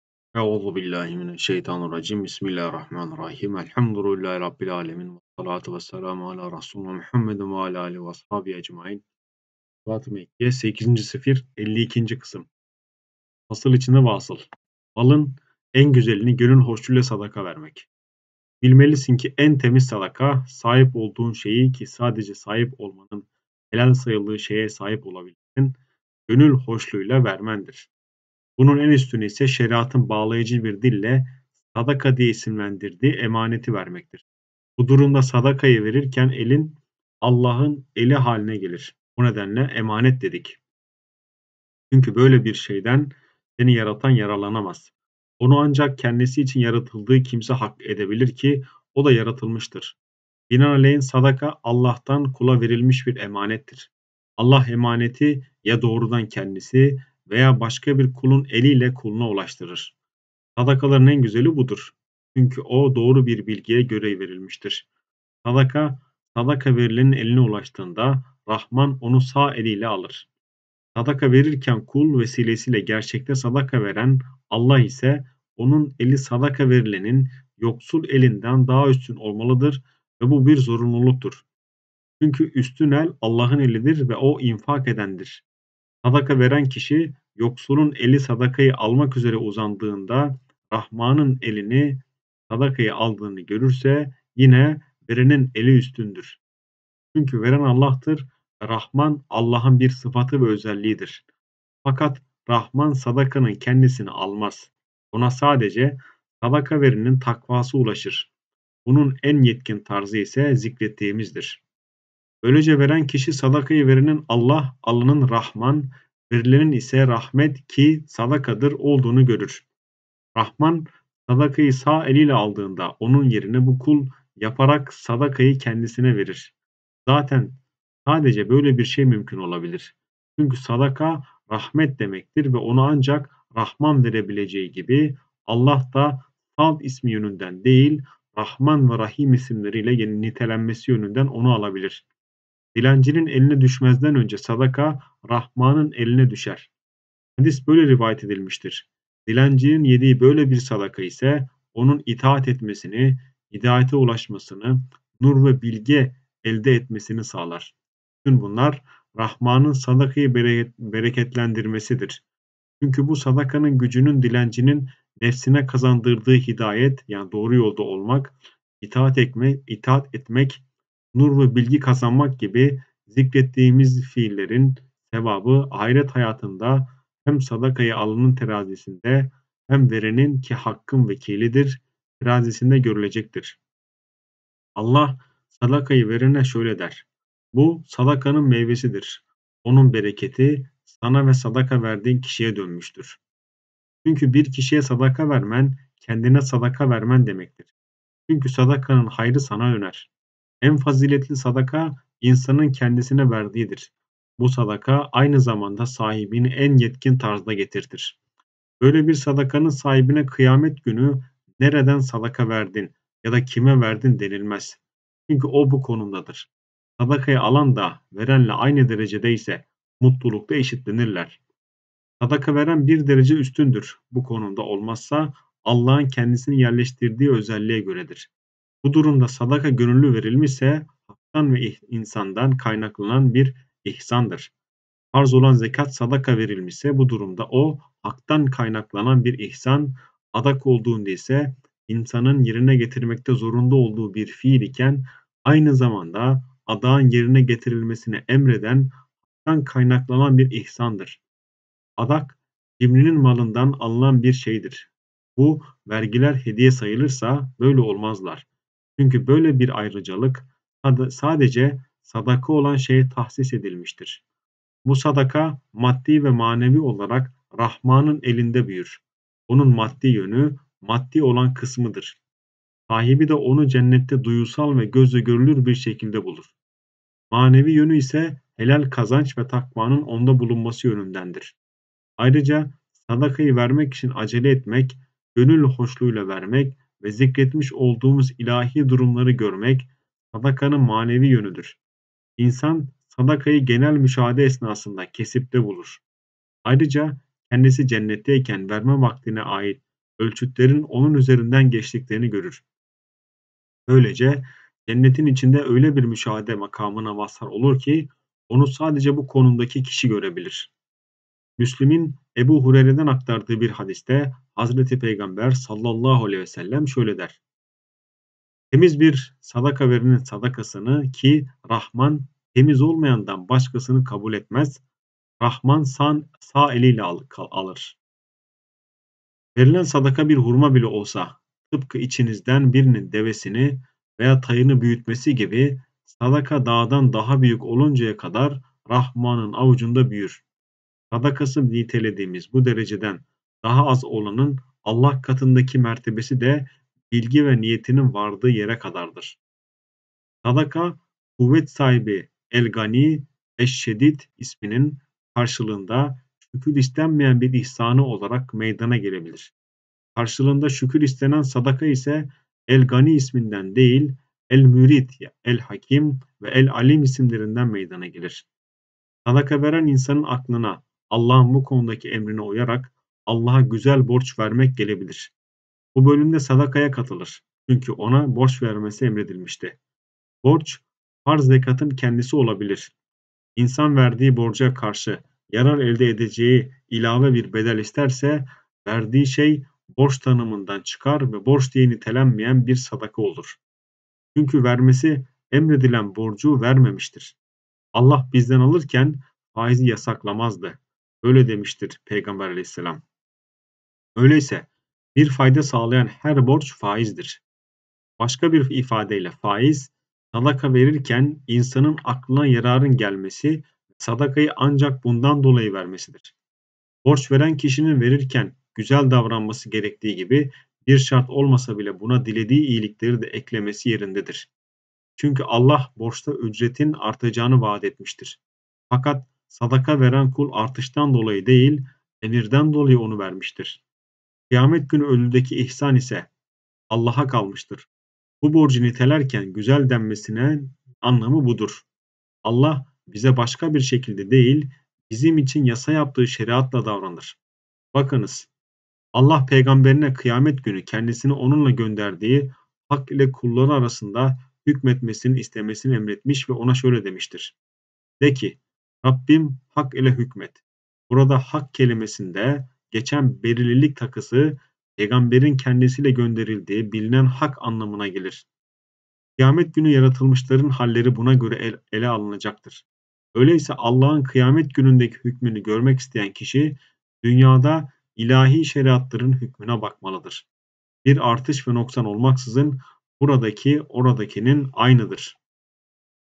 Fütuhât-ı Mekkiyye - Malın En Güzelini Sadaka Vermek (8. Sifr 52. Kısım) - 8
YETMİŞİNCİ BÖLÜM
Zekatın Sırları

🕘Okunan Yerler:

Malın En Güzelini Gönül Hoşluğuyla Sadaka Vermek

Fütuhât-ı Mekkiyye Sesli Kitap (Tamamı)